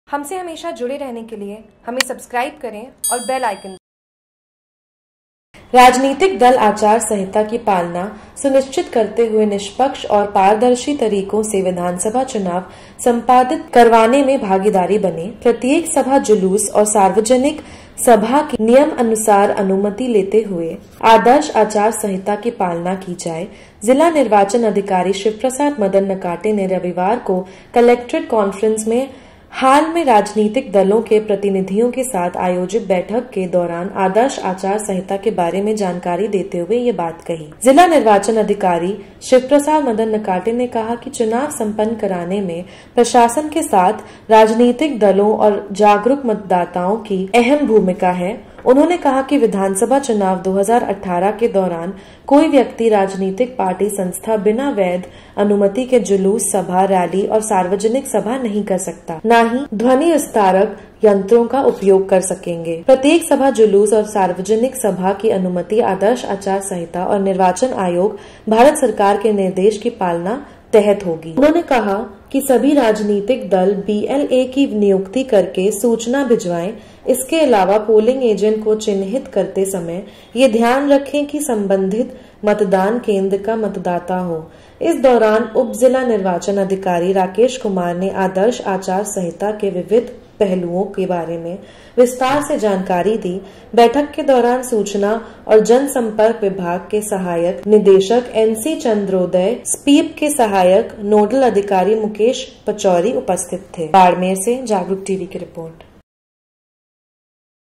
JAGRUK TV : बाड़मेर : राजनीतिक दलों के प्रतिनिधियों को दी आचार संहिता की जानकारी।
राजनीतिक दल आचार संहिता की पालना सुनिश्चित करते हुए निष्पक्ष एवं पारदर्शी
तरीके से विधानसभा चुनाव संपादित करवाने मे भागीदार बनें। प्रत्येक सभा, जुलूस एवं
सार्वजनिक सभा की नियमानुसार अनुमति लेते हुए आदर्श आचार संहिता की पालना की जाए।
जिला निर्वाचन अधिकारी शिवप्रसाद मदन नकाते ने रविवार को कलेक्ट्रेट कांफ्रेस हाल मे
राजनीतिक दलॉ के प्रतिनिधियों के साथ आयोजित बैठक के दौरान आदर्श आचार संहिता के बारे
मे जानकारी देते हुए यह बात कही।